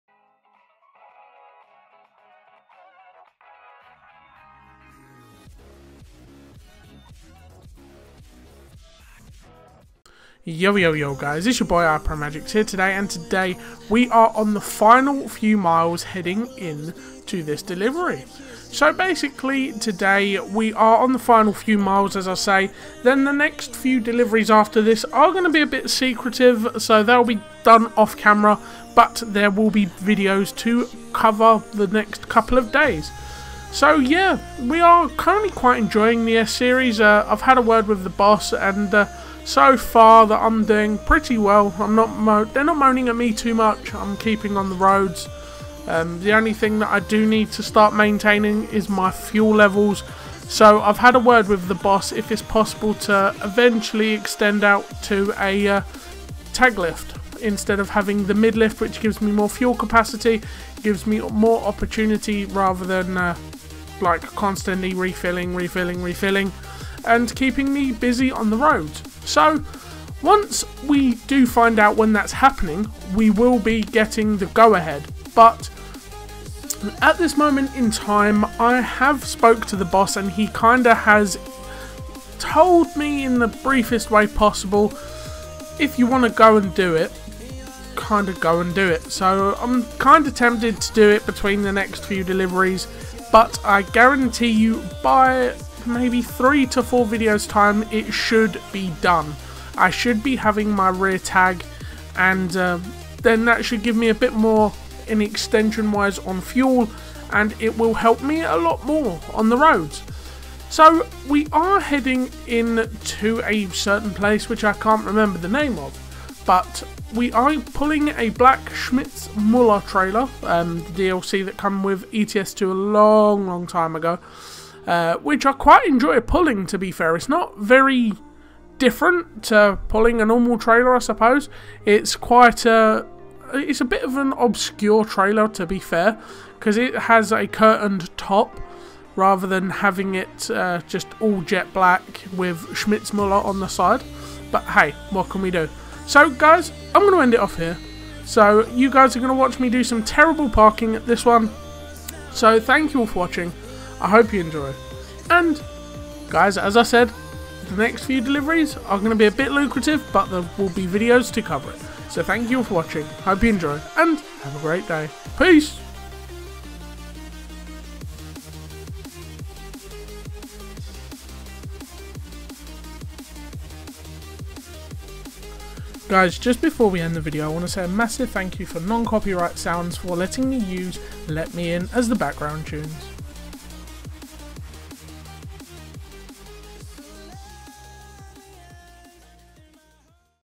Calm Before The Storm! (RT1MM EP32!)
Time For Road To One Million Miles Again!
Today We are moving some food heading to sweden but we are on some major loads so videos will be fun for the next few day!
In My New Scania S Series Also Known As WhiteHart (WHL)